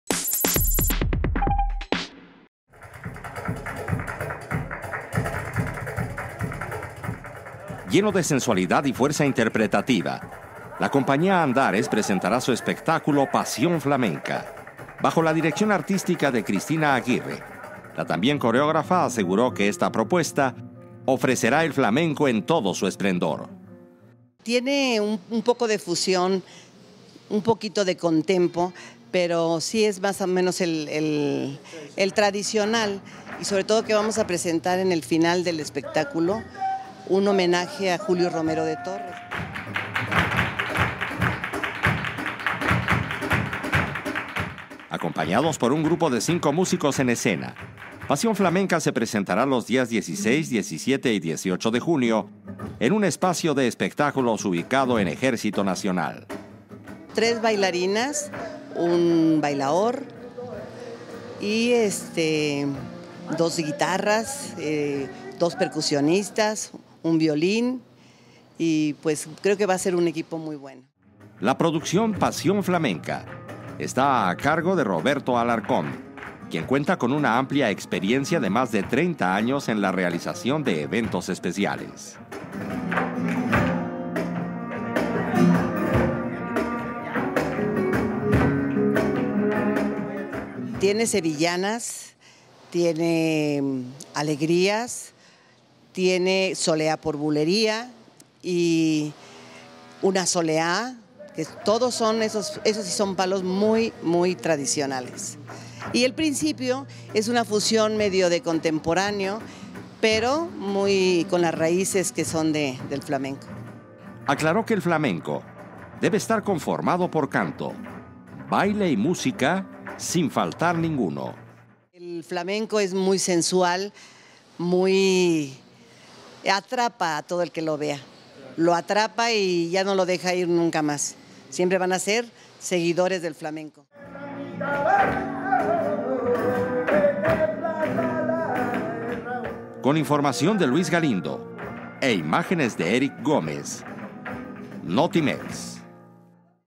Estrenará la compañía Andares su espectáculo "Pasión Flamenca"
La compañía Andares presenta el espectáculo "Pasión Flamenca" con la dirección artística de Cristina Aguirre